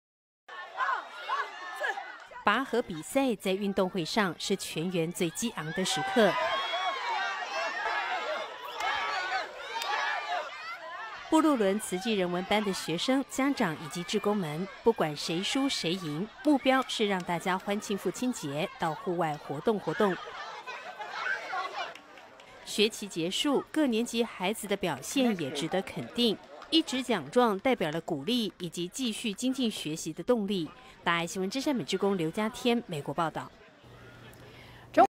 20190623紐約慈濟布祿崙人文班結業式
位在美國紐約的慈濟 布祿崙聯絡點，假日為人文班舉辦結業式，由於六月也適逢慶祝美國父親節，因此志工們規畫了運動會，大家歡喜參與。

拔河比賽，在運動會上，是全員最激昂的時刻。

布祿崙慈濟人文班的學生、家長、以及志工們，不管誰輸誰贏，目標是讓大家歡慶父親節，到戶外活動活動。

學期結束，各年級孩子的表現也值得肯定，一紙獎狀代表了鼓勵以及繼續精進學習的動力。

劉家添 真善美志工-美國報導